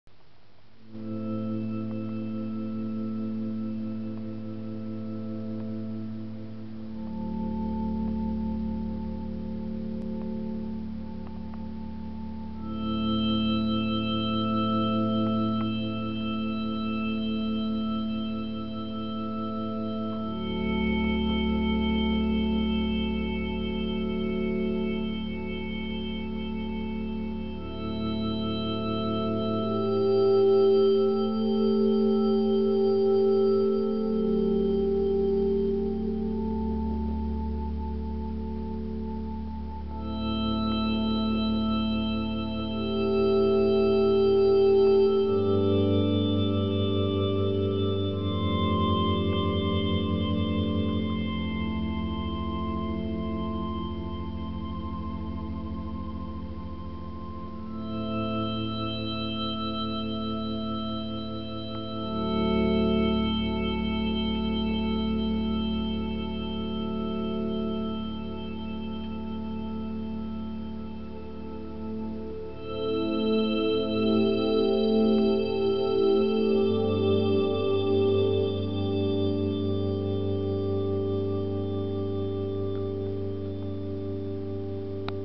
Evening of Waves on my Microkorg XL
A very small demonstration of the "digital" side of microKORG XL I made for a friend. It is perfectly able to imitate those 80's digital KORG type of sounds (DW-8000, DS-8, DSS-1). A little bit of modulating delay is introduced in the middle of the video. Maybe some glitched drums would work over this and I could turn this into a complete song. I just haven't had the time to do so.